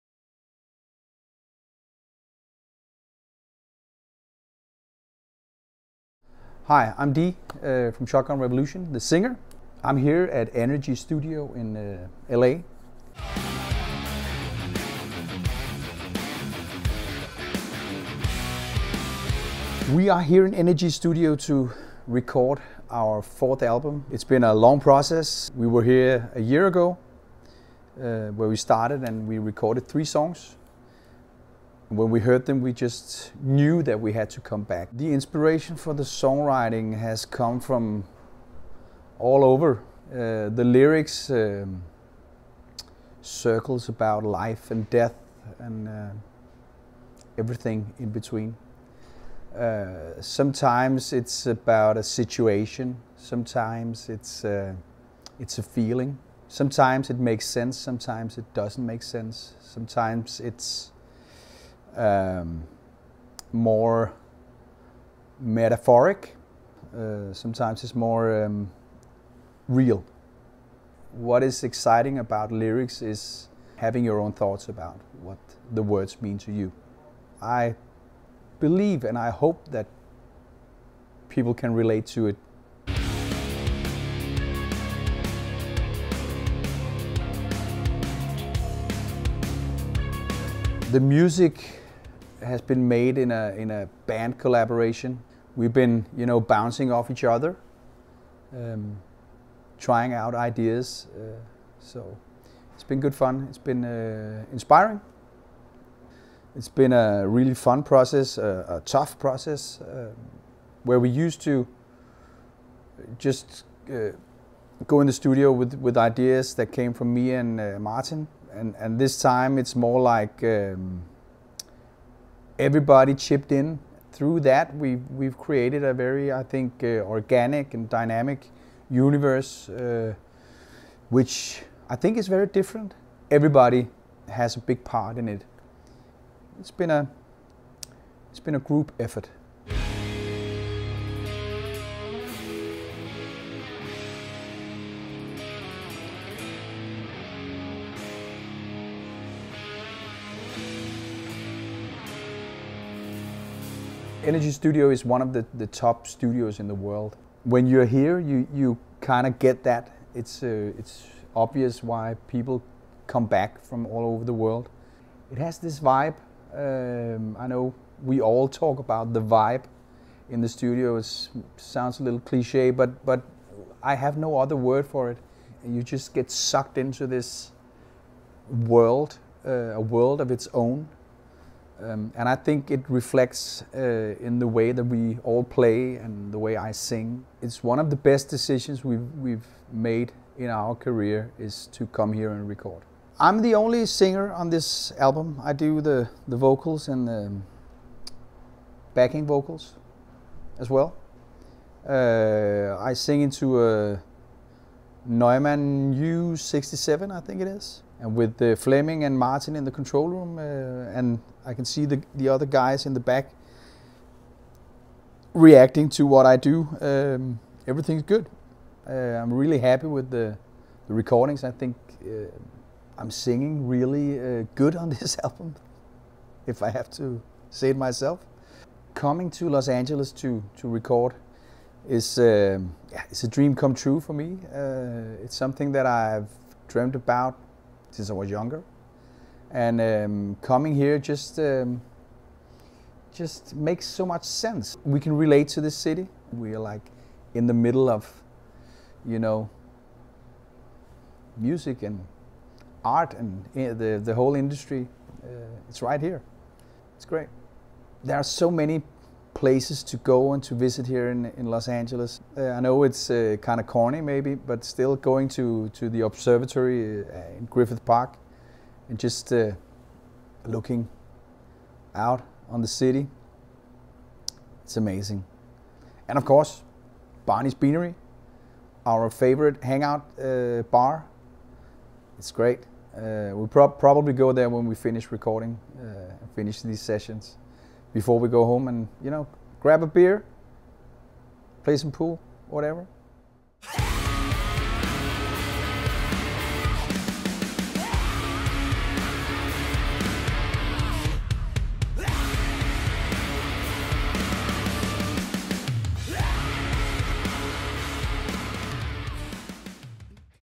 DITLEV "DEE" ULRIKSEN (from NRG STUDIOS, Los Angeles)
Dee shares some thoughts about the making of SR 4

Filmed in NRG Recording Studios march 14. 2019.